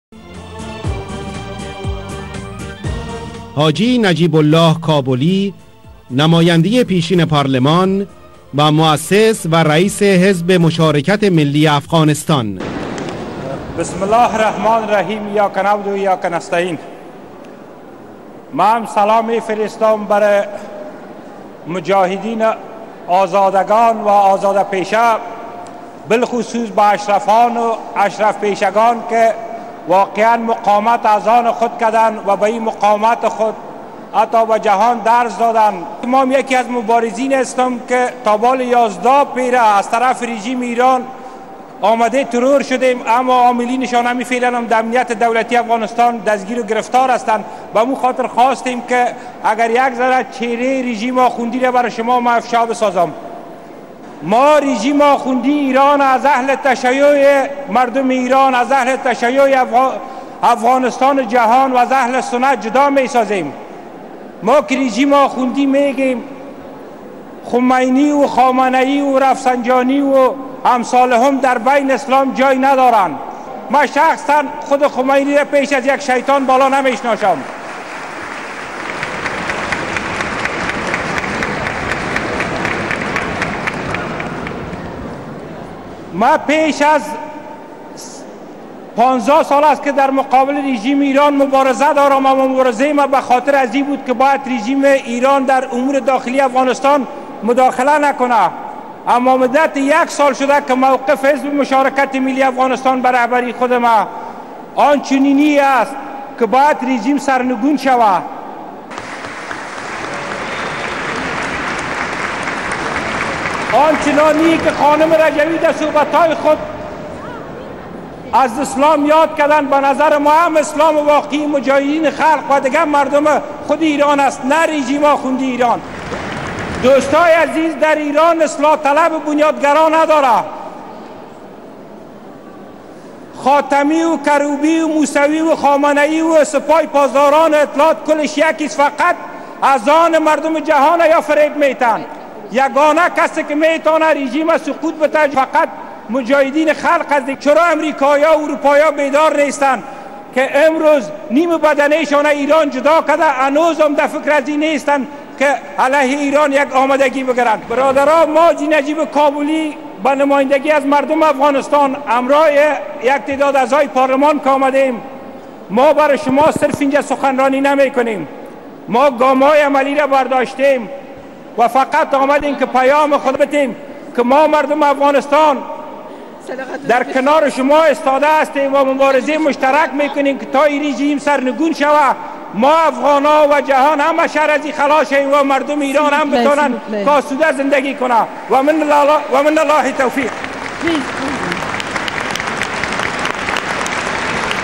Najeeb kabuli .wmv
در این سیمینارکه در پاریس برگزارگردیده بود مخالفین رژیم ولایت مطلقه فقیه به عموم مردم جهان اعلان کردند که این رژیم مستبد را مردم ایران نمیپذیرند وباید ملت های آزادیخواه جهان ایرانیان را دراین مبارزه یاری کنند